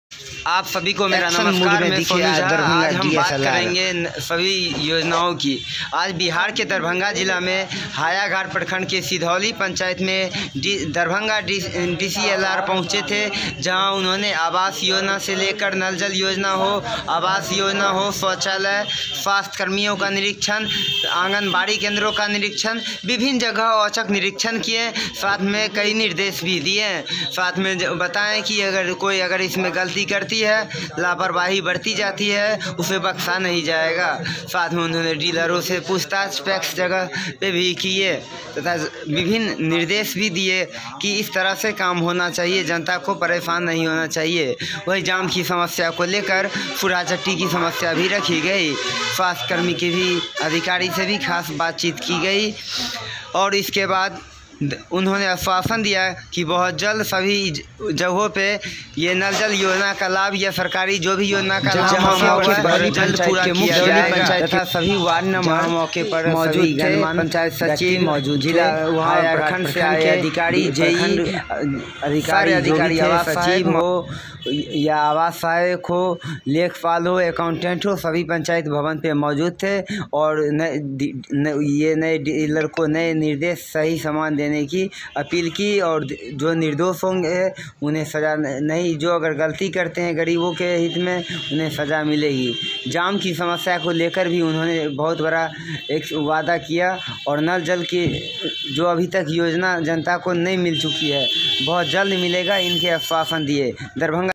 एक्शन मूड में दिखे दरभंगा डीएसएलआर सिधौली पंचायत की जांच
बिहार के दरभंगा जिला के हायाघाट प्रखंड के सिधौली पंचायत में डीसीएलआर दरभंगा पहुंचे डोली पंचायत में सभी औचक निरीक्षण किया जहां की खास चिंता आंगनवाड़ी केंद्रों से लेकर नल जल योजना हो या टैक्स का मामला हो या डीलर का मामला और सभी जगह का जांच की जांच के दौरान उन्होंने कई निर्देश लीजिए